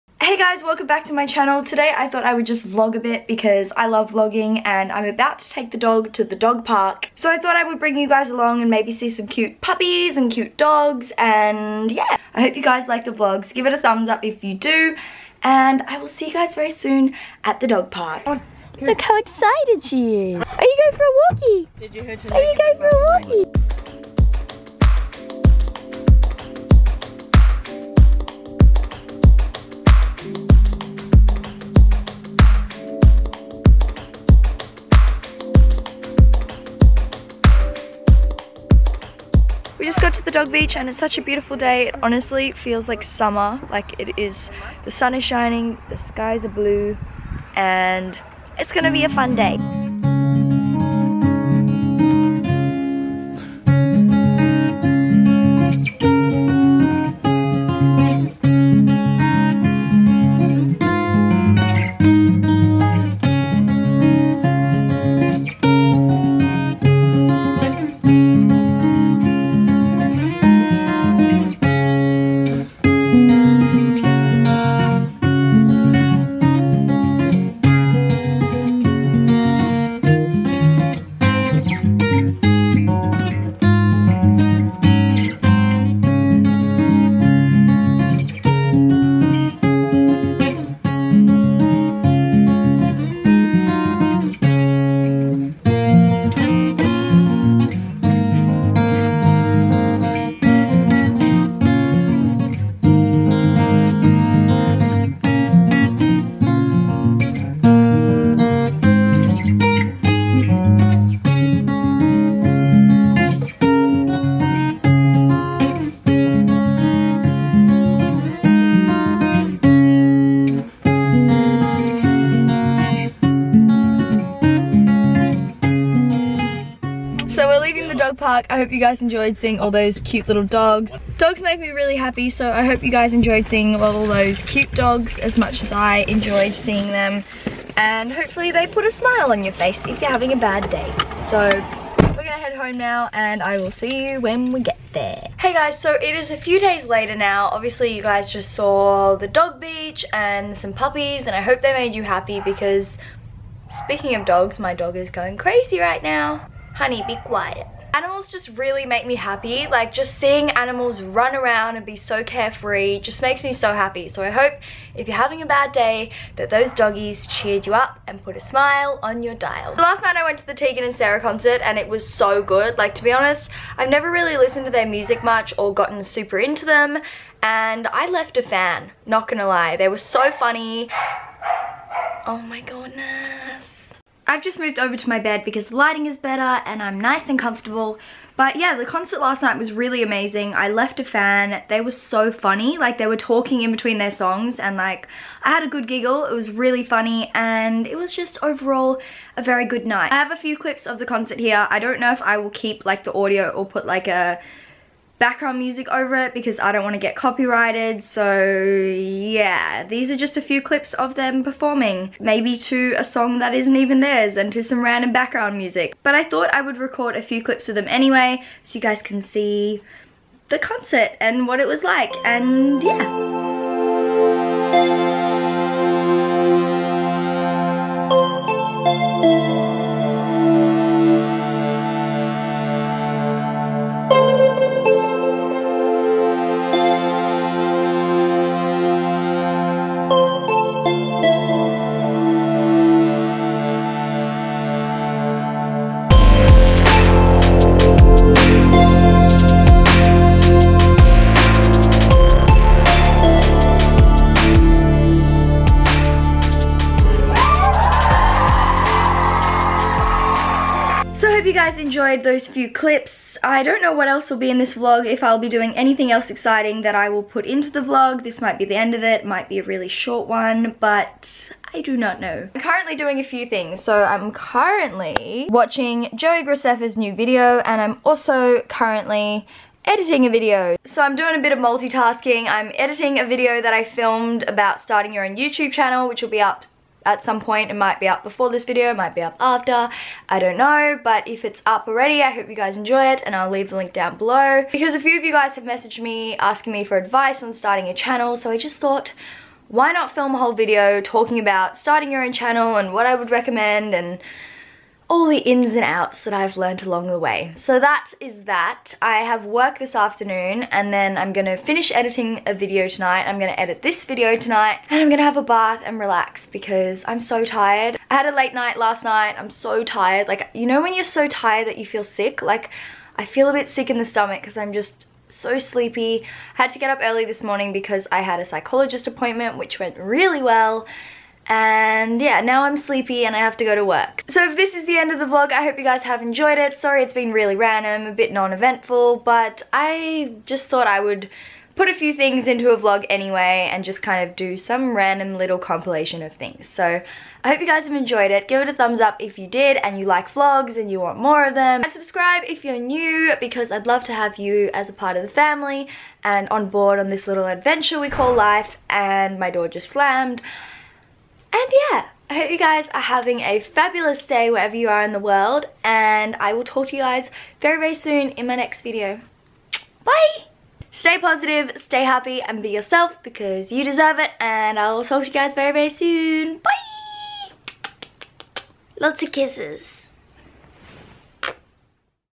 A VLOG OF DOGS + Tegan and Sara Concert
HOLLA (CITY OF SQUALA!)
I thought I would just upload a lil vlog that I put together a little while ago. I know the Tegan and Sara concert was a while back but I thought I would upload it anyway :)

I hope all these cute lil pups put a smile on your dile :) I had so much fun at the dog beach and at the Tegan and Sara concert, I couldn't not share it with you!

PS I fricken love vlogging so I hope you guys enjoy them as much as I enjoy making them :)

i love you all lots, thanks for being a bunch of legends and watching my shit!

xxxxx